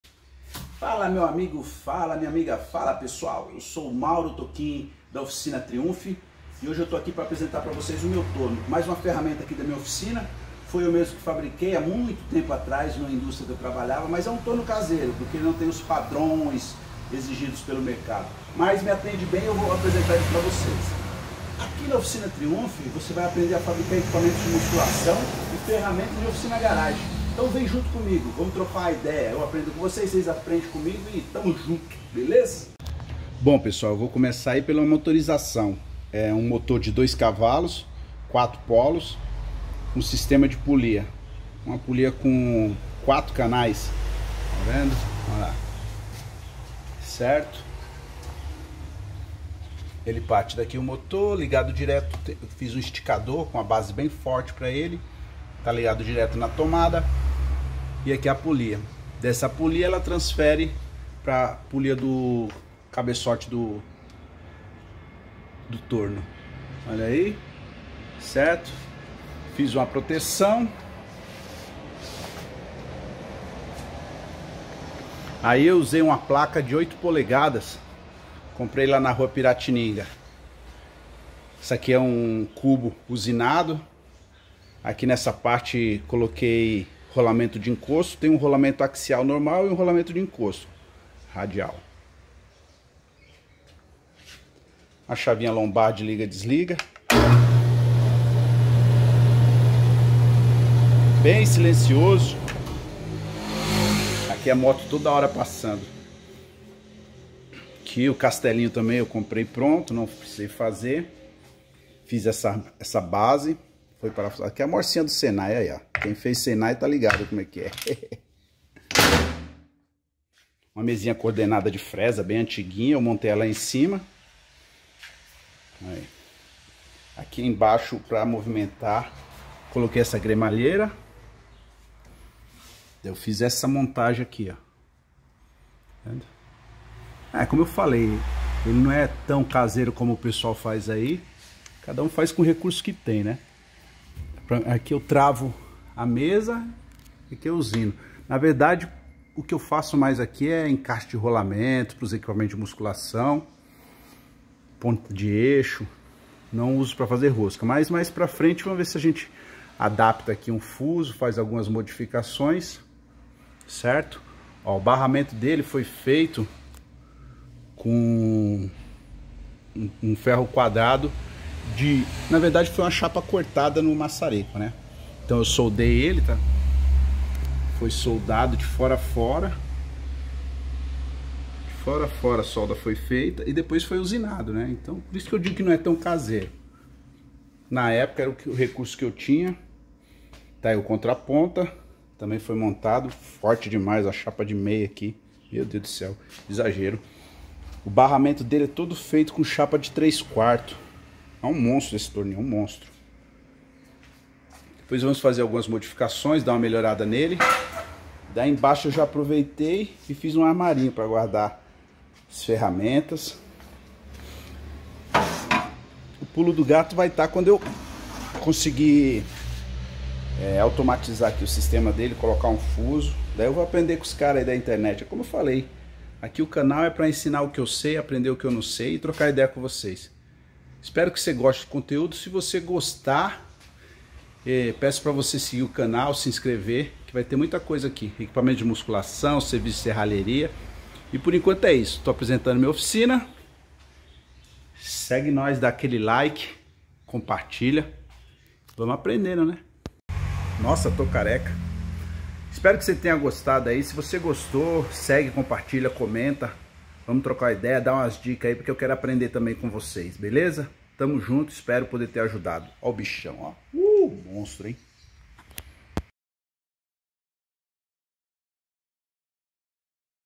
Como fabricar um Torno Caseiro/ Lathe DY home.
Apresentação do Canal, mostrando torno "caseiro".